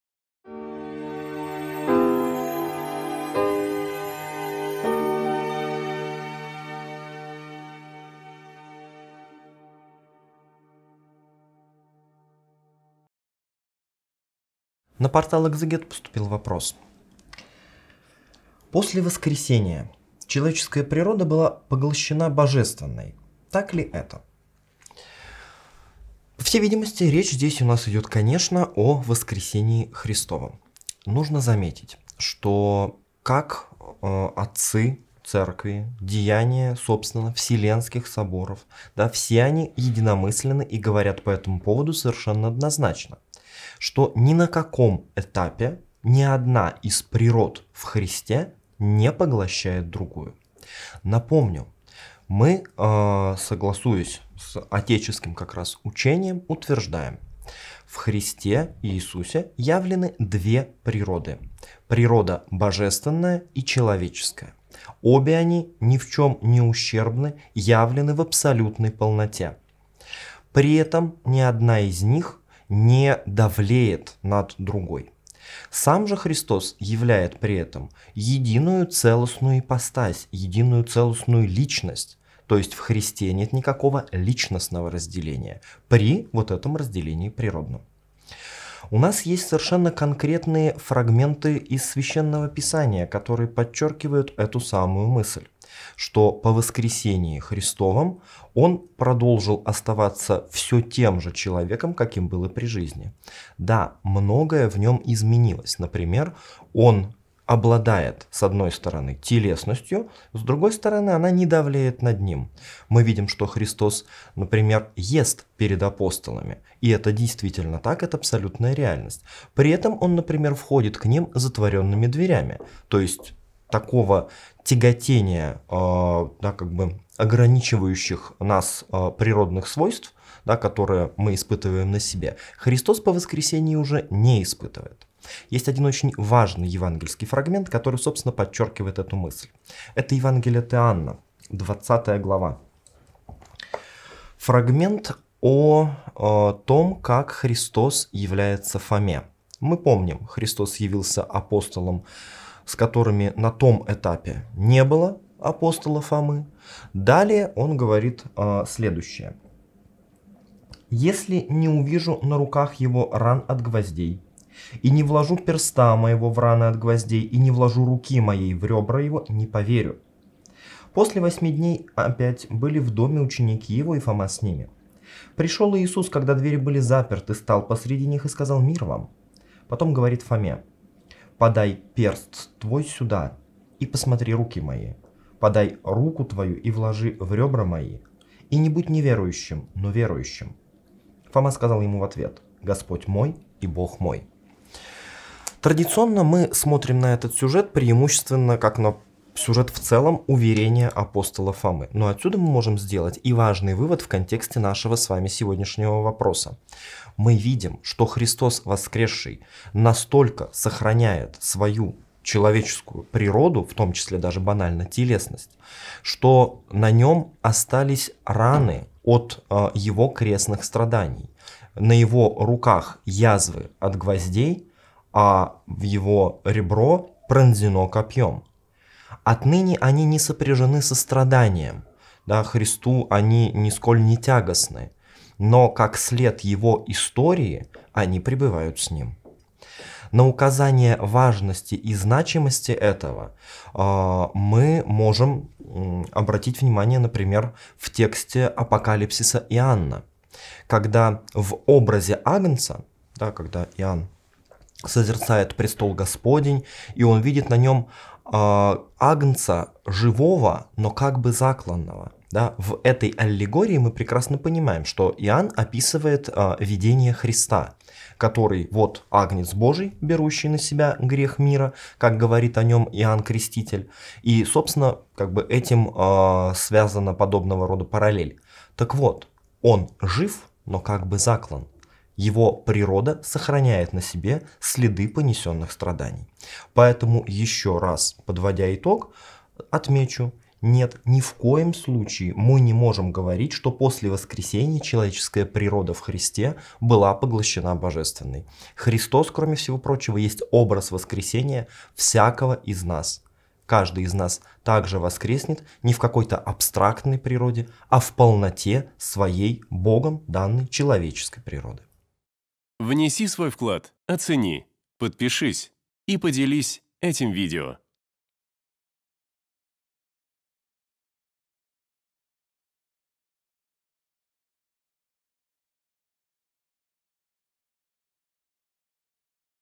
ПОСЛЕ ВОСКРЕСЕНИЯ ЧЕЛОВЕЧЕСКАЯ ПРИРОДА ХРИСТА БЫЛА ПОГЛОЩЕНА БОЖЕСТВЕННОЙ ? о. Александр Сатомский
Команда Экзегет.

На библейском портале Экзегет вместе со  священником Александром Сатомским, преподавателем кафедры теологии Ярославской Духовной Семинарии, отвечаем на вопрос: "После воскресения человеческая природа Христа была поглощена Божественной?"

🔸  Две природы Христа явлены в абсолютной полноте
🔸  Единая целостная личность
🔸  Свидетельства о сохранении человеческой природы
🔸  Образ воскресения

📜 Читать Евангелие от Иоанна главу 20, стих 27 (с толкованиями)